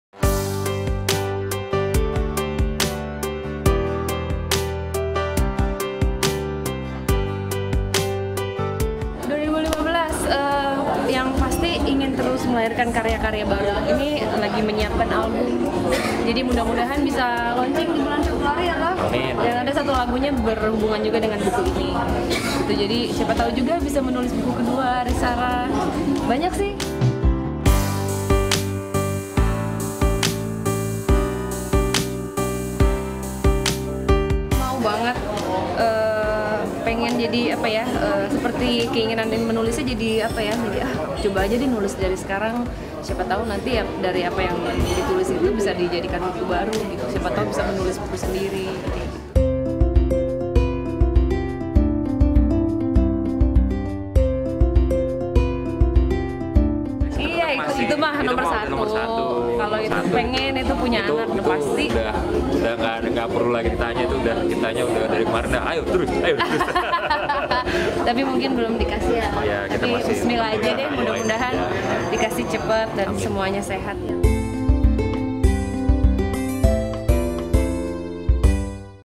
Ini Harapan Demian & Sara Wijayanto di tahun 2015
Pasangan Demian Aditya &amp;amp; Sara Wijayanto memiliki sejumlah harapan jelang pergantian tahun menuju 2015. Seperti apa harapan keduanya? Yuk, simak selengkapnya dalam tayangan yang satu ini.